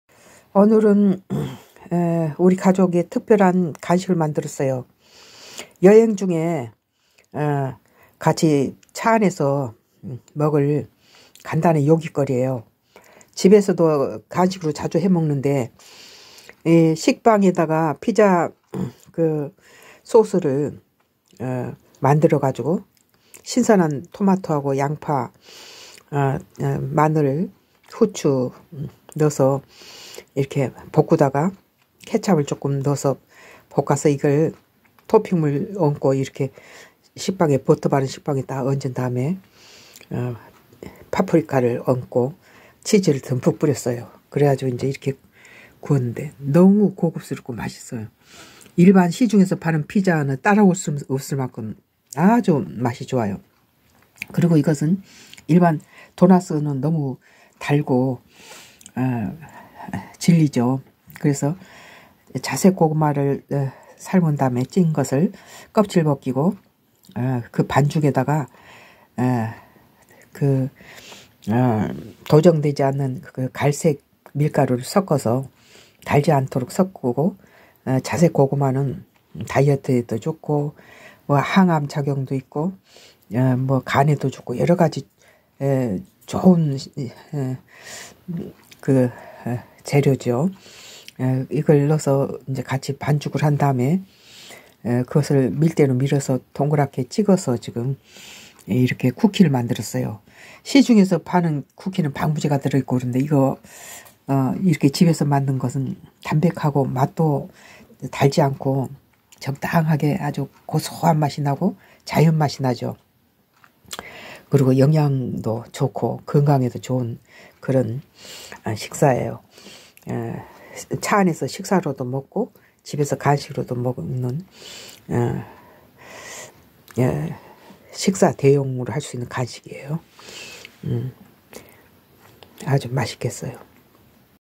MAMA FOOD 엄마표! 자색고구마 쿠키,피자빵~~!@sungodcross
엄마표! 자색고구마 쿠키,피자빵~~!

외출할 때는~~우리 가족은~~코로나 때부터~~차 안에서 식사를 하는 습관이 ~~생겼다~~!

그래서 식당이나 마트에서 포장하여 오거나~~미리 차 안에서 먹을 도시락이나~~간단한 간식이나~~음료를 집에서 만들어 간다

오늘은! 자색고구마를 삶아서~~도나쓰 가루와 갈색 밀가루를 ~섞어서 쿠키를 만들고~~~!

식빵을~~버터발라 구운 후~~~!
양파와 생토마토에 후추 마늘을 버터에 함께 볶은 쏘스를~~

구운 식빵 위에 바르고~~그 위에 쏘스를 얹은 다음~치즈를~ 듬뿍 뿌려서 구우면 ! 맛있는 피자빵이 된다`~~!
시중에 파는~~ 피자와는 비교가 안될 만큼 맛있다~~~!@sungodcross